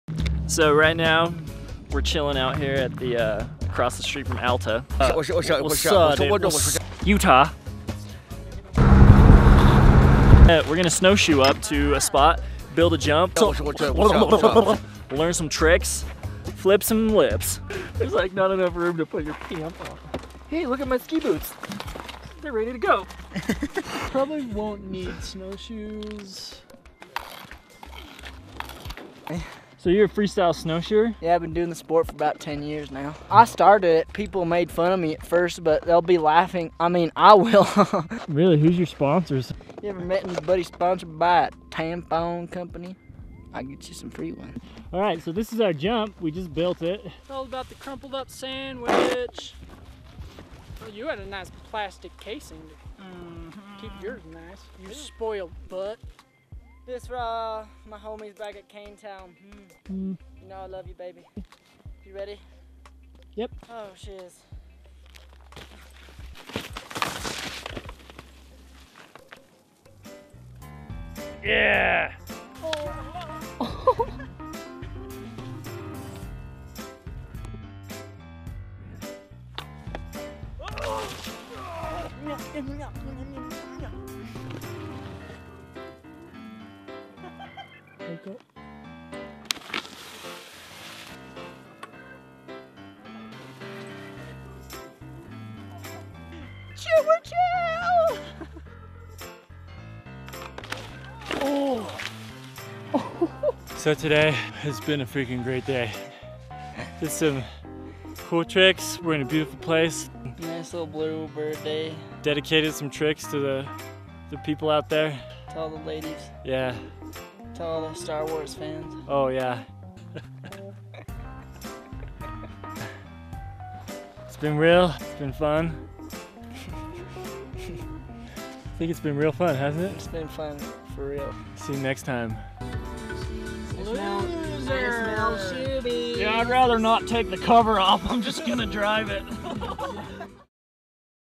Back County Booter
Little cottonwood canyon skiing is the beez keez! We may not be that good, but we know how to party. Check it out!